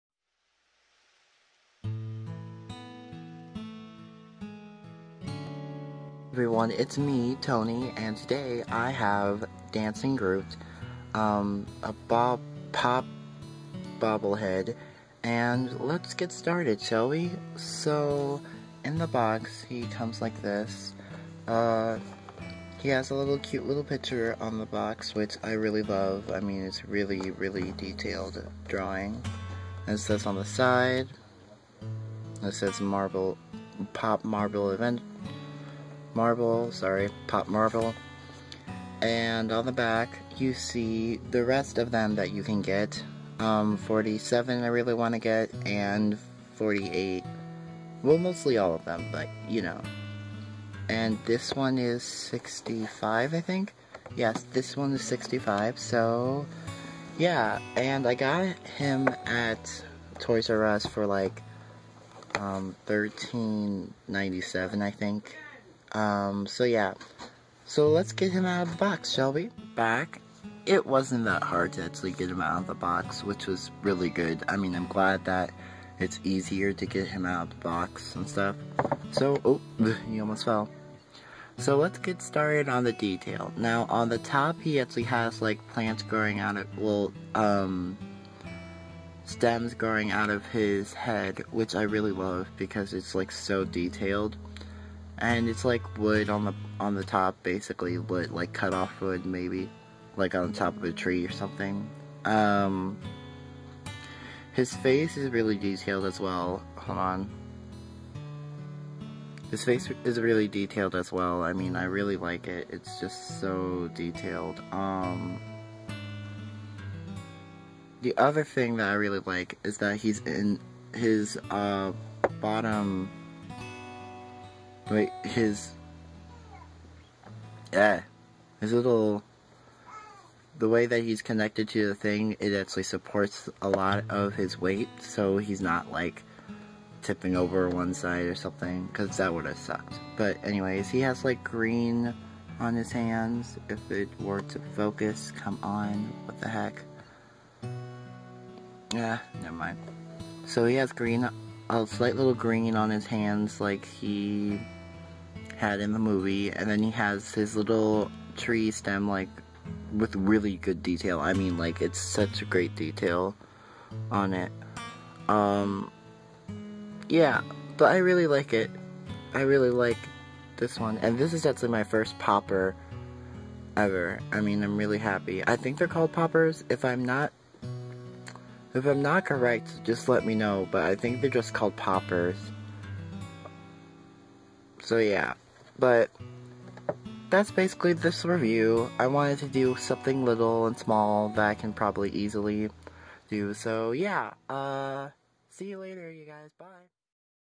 Pop dancing Groot review
Just showing you my new bobblehead from the movie legend of the Guardians